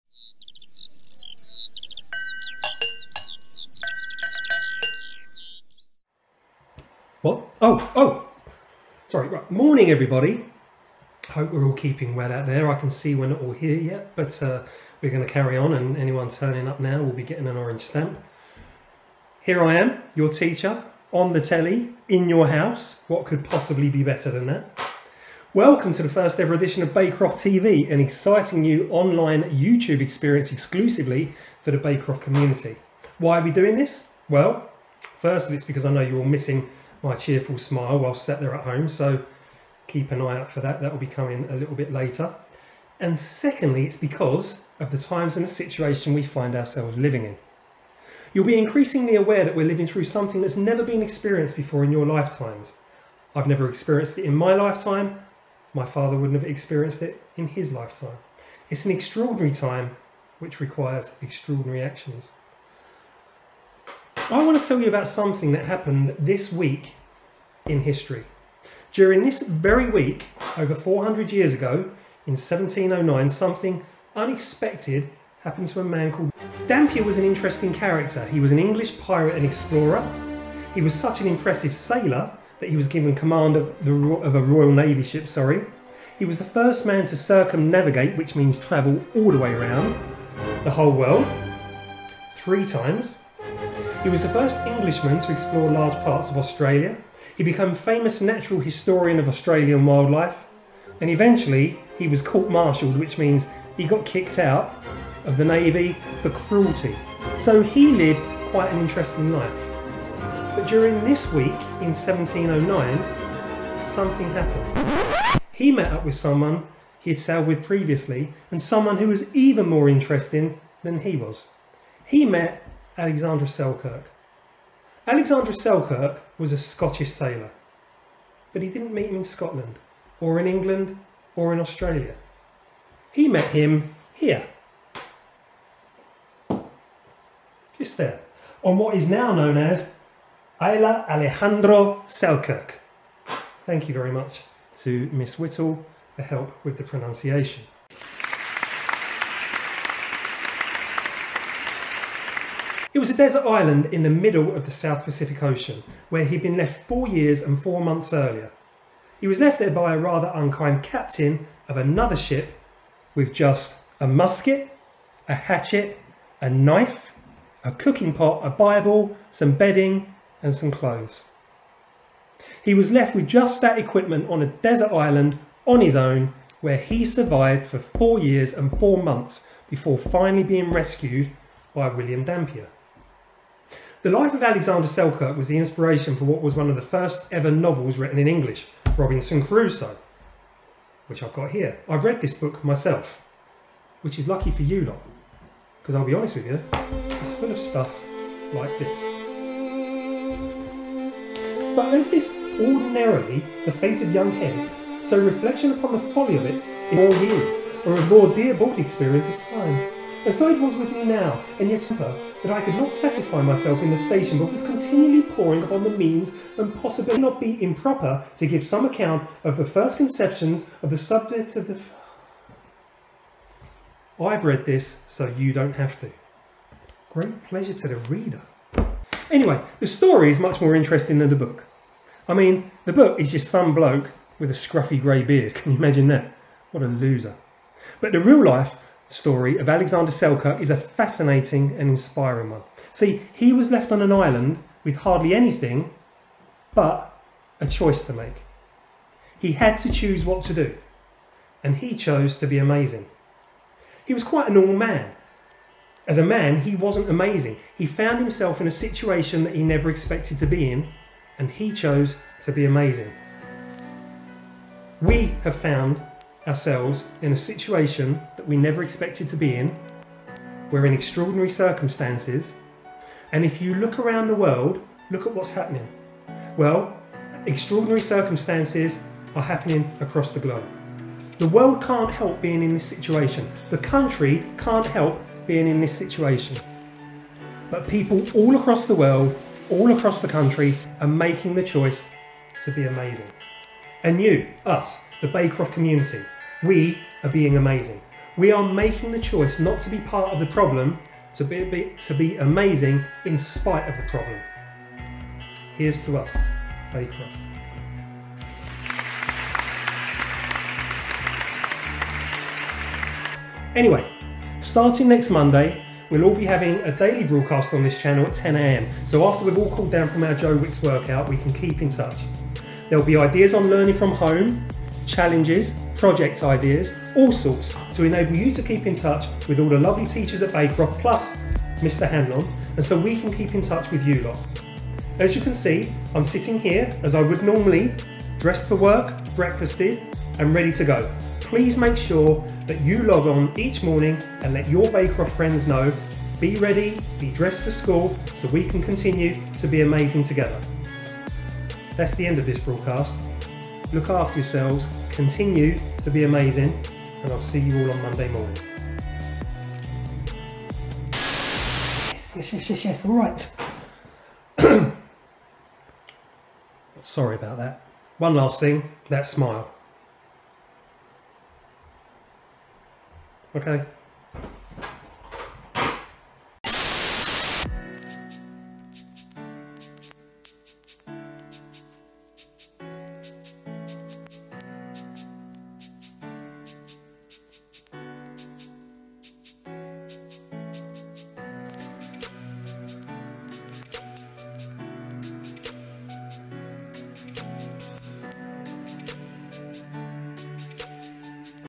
Baycroft TV [OFFICIAL] Pilot Episode
Hi Baycroft! Today Mr Mitchell and Mr Kiff lead us into Baycroft's newest venture; television. You really thought we would leave you alone to play Fortnite and dance to Tik Toks all day? Of course we wouldn't!

We will be publishing every weekday at 10am, so do your Joe Wicks workout and cool down before you tune in. We look forward to seeing you for a daily dose of stuff to do during lockdown, with the chance to win some *pretty significant* prizes for good effort!